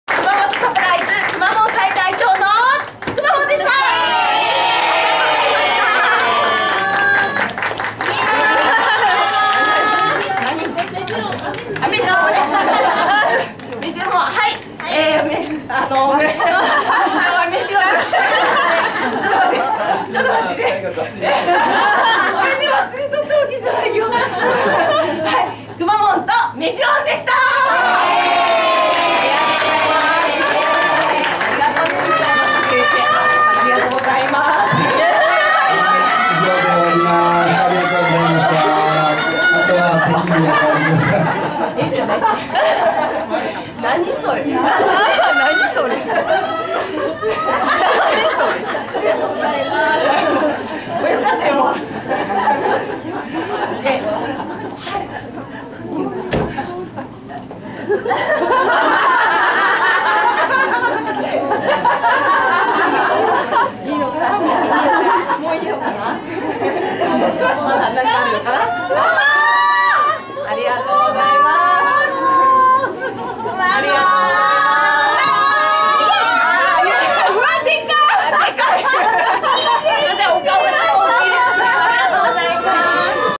170316_くまモン[05]めじろん紹介さす
「「熊本・大分復興支援フェア」支援金贈呈式」で、みちのく夢プラザに出没した『くまモン』。『めじろん』も紹介するよう促す。